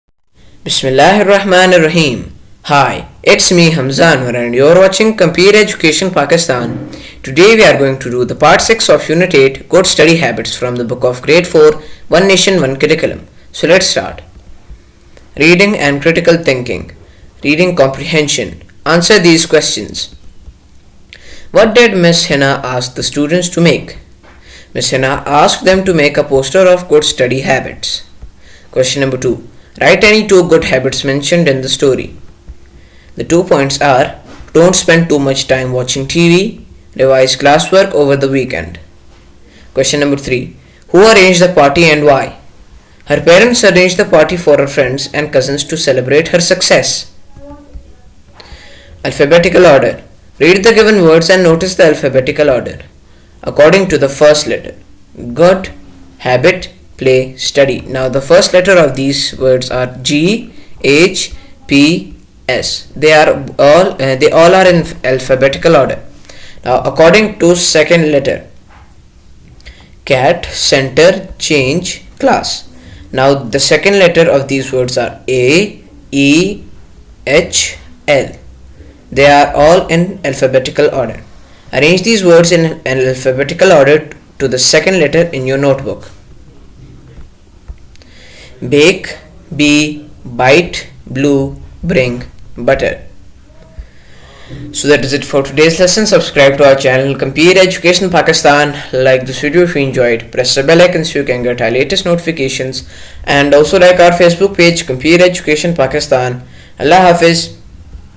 Unit 8, Good study Habits, Part 6, Activities, Grade 4, One Nation One Curriculum
In this lesson, students will do the activities from Unit 8, Good study habits from the book Grade 4, One Nation One Curriculum.